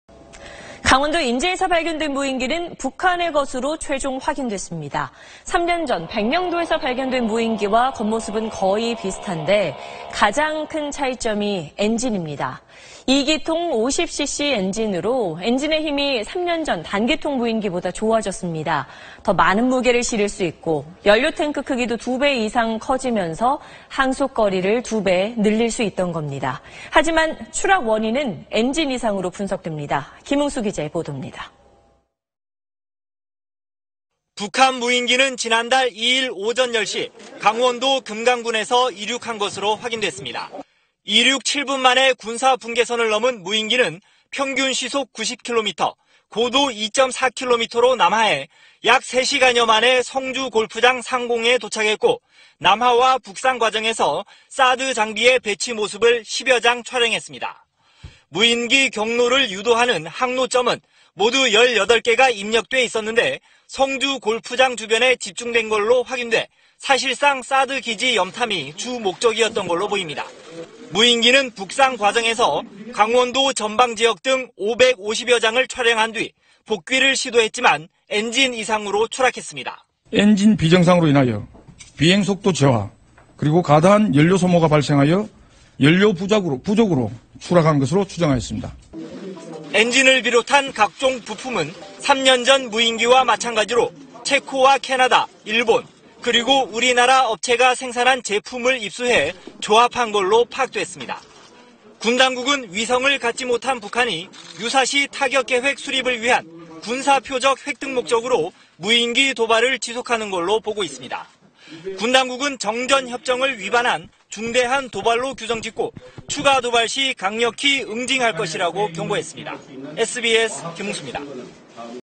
'사드 염탐' 北 무인기…"엔진 이상 추락" / SBS
강원도 인제에서 발견된 무인기는 북한의 것으로 최종 확인됐습니다. 3년 전 백령도에서 발견된 무인기와 겉모습은 거의 비슷한데 가장 큰 차이점이 엔진입니다. 2기통 50CC 엔진으로 엔진의 힘이 3년 전 단기통 무인기보다 좋아졌습니다. 더 많은 무게를 실을 수 있고, 연료탱크 크기도 2배 이상 커져서 항속거리를 2배 늘릴 수 있던 겁니다. 하지만 추락 원인은 '엔진 이상'이었던 걸로 분석됩니다.

김흥수 기자의 보도입니다.